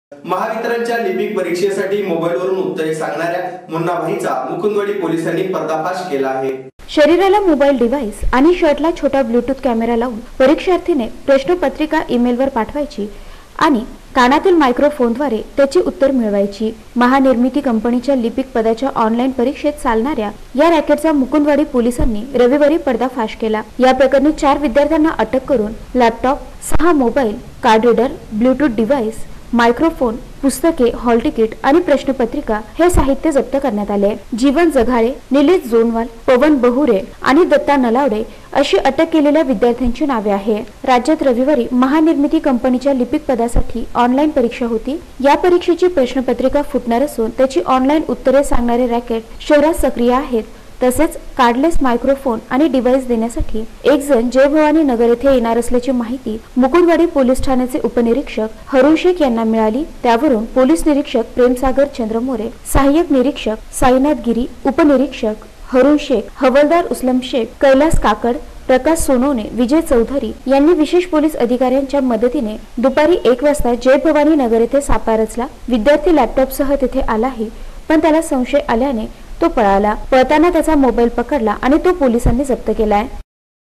महानिर्मिती लिपिकाच्या परीक्षेत मोबाईलवरून उत्तरे सांगणारे मुकुंदवाडी
Aurangabad : महानिर्मिती लिपिकाच्या परीक्षेत मोबाईलवरून उत्तरे सांगणारे मुकुंदवाडी पोलिसांच्या ताब्यात